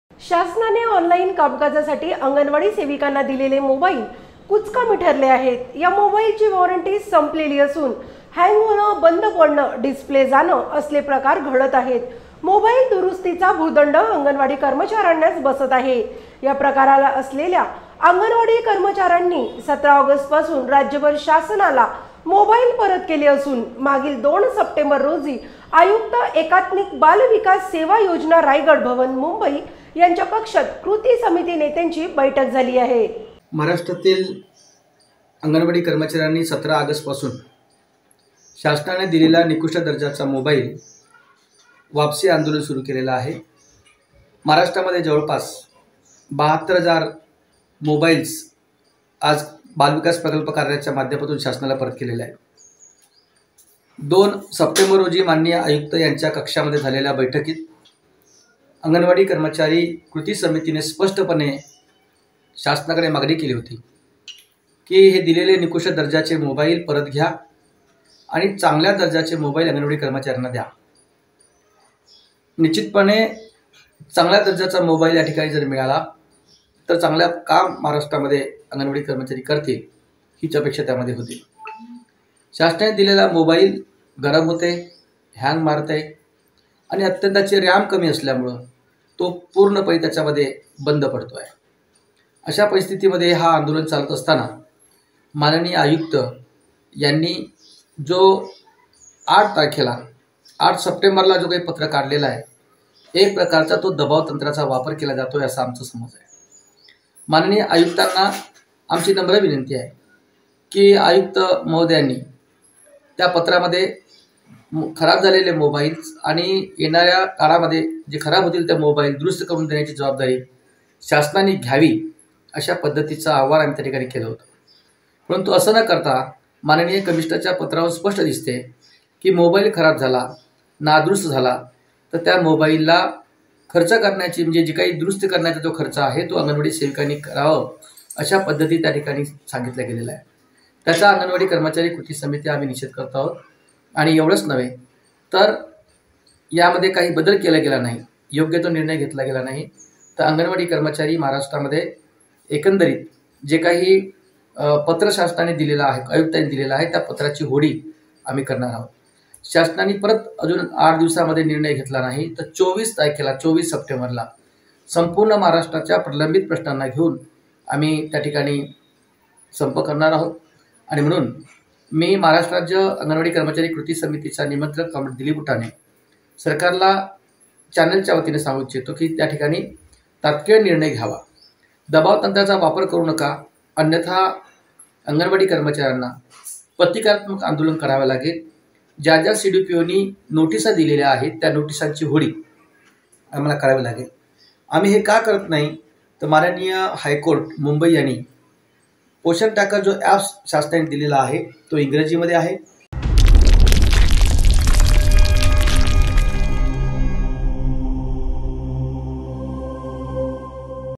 शासनाने योग्य निर्णय घेतले नाहीतर २४ सप्टेबर रोजी देशव्यापी संप
आयुक्तानी दिलेले मोबाईल पञाची अंगणवाडी कर्मचारी करणार होळी